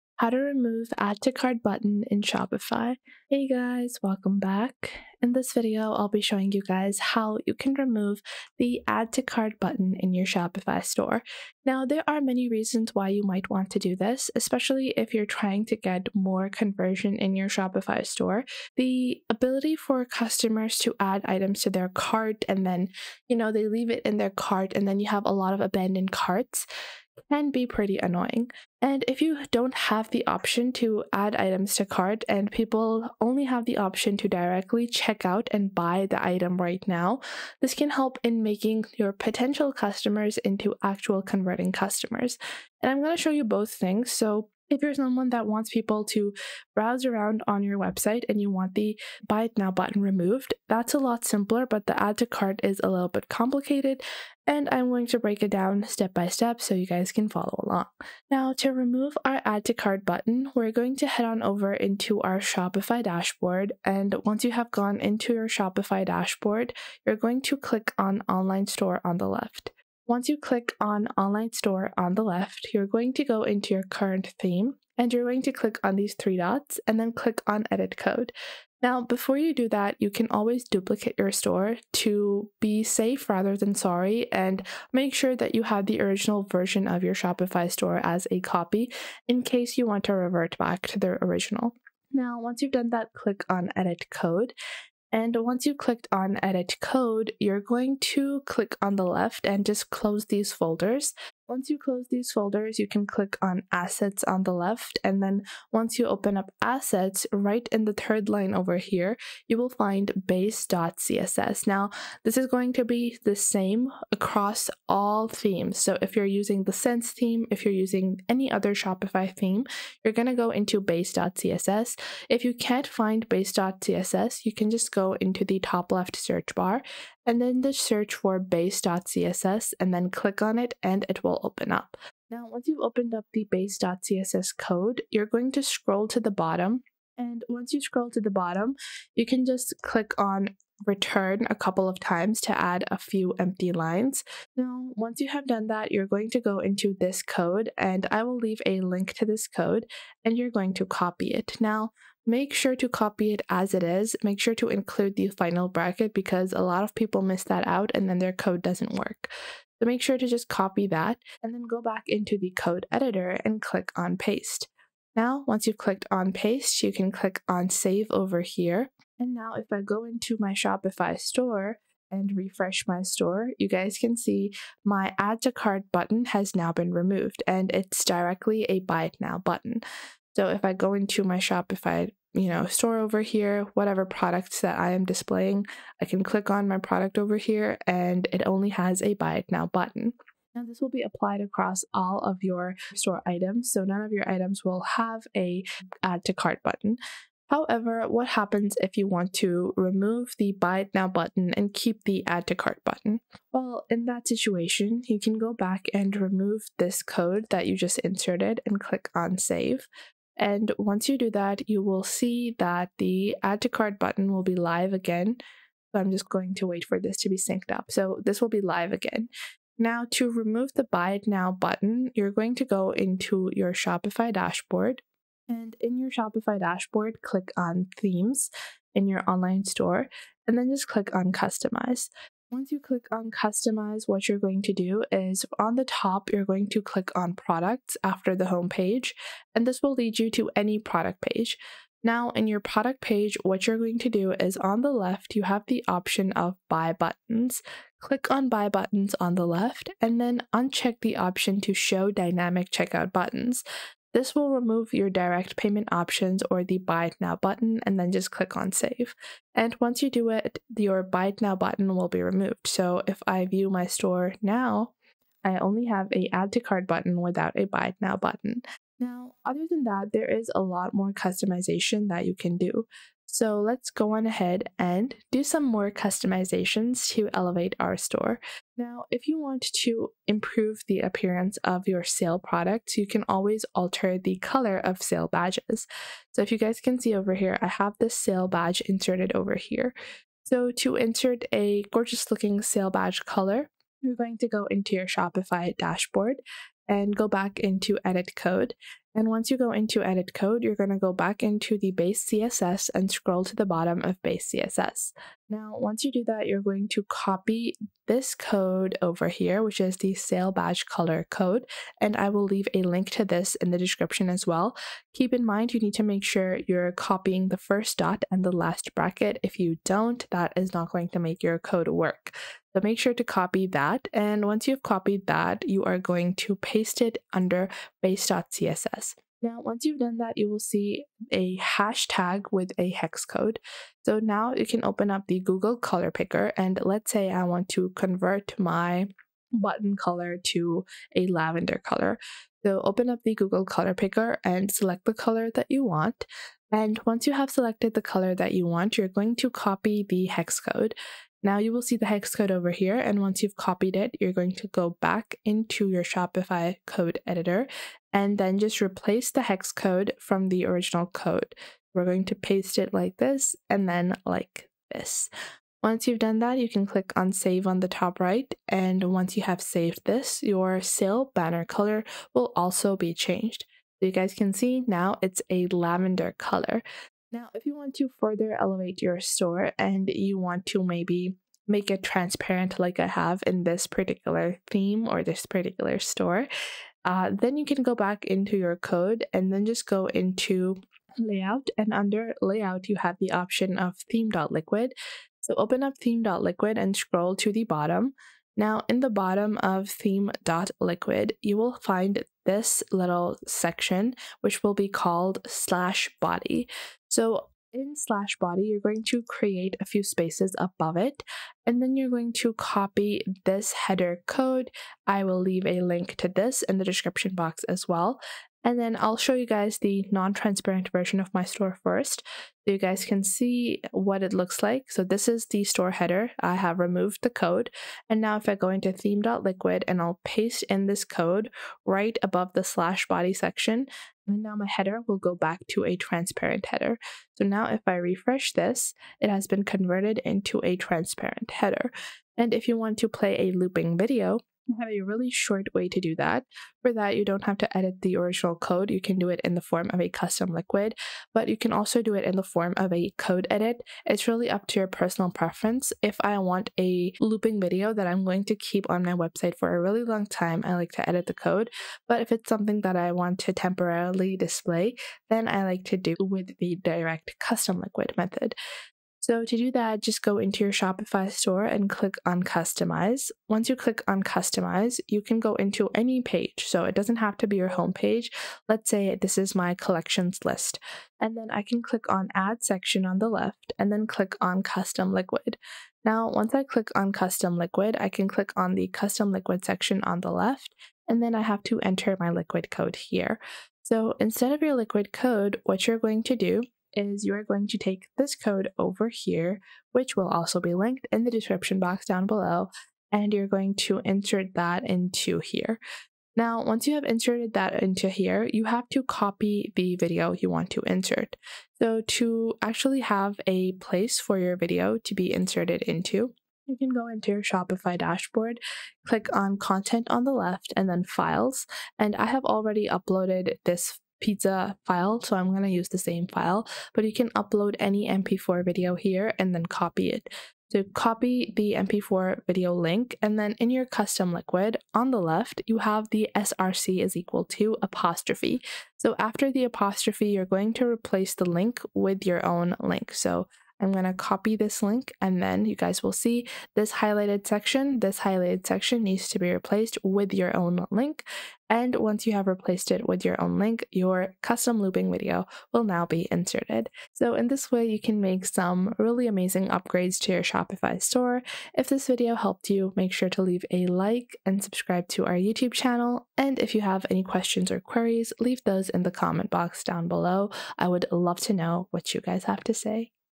How To Remove Add To Cart Button In Shopify 2026
In this video we show you How To Remove Add To Cart Button In Shopify. This video will cover everything that you need to know and help you get started with this software.